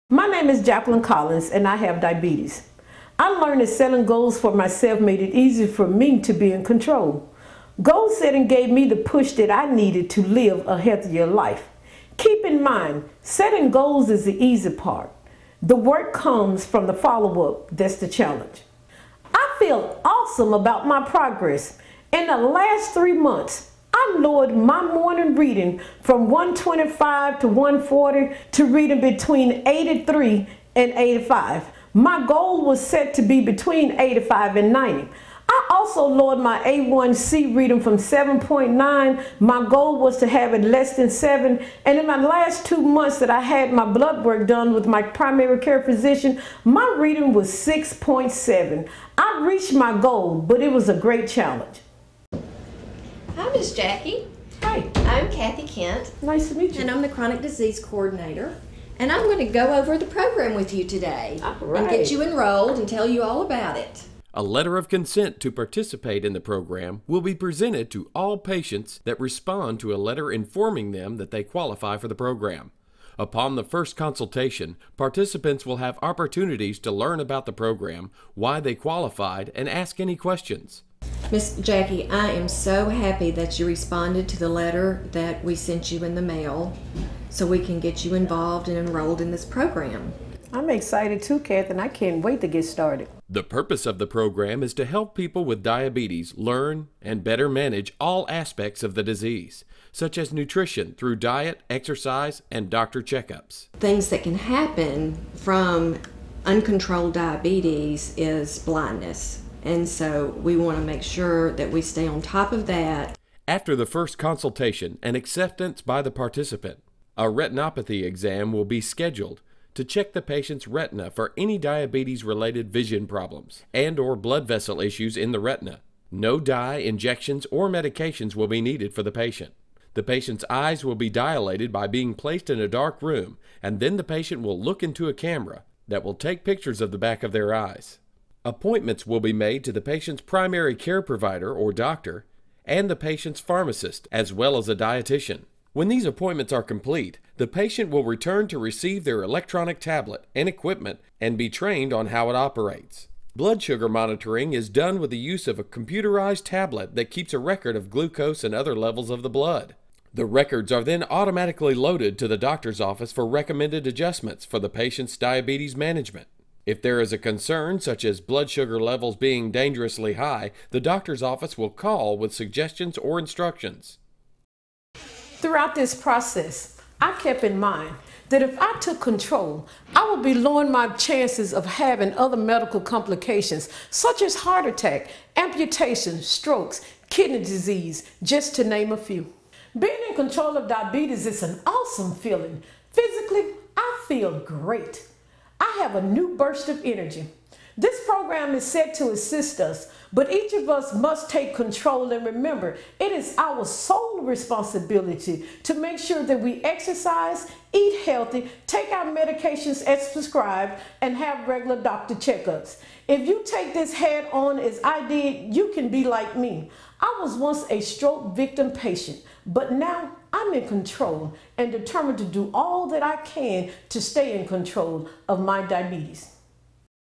Diabetic Telehealth Network Meet Jackie Collins
Meet Jackie Collins and listen to her testimony since joining the Diabetic Telehealth Program at Sunflower Clinic in Ruleville, MS.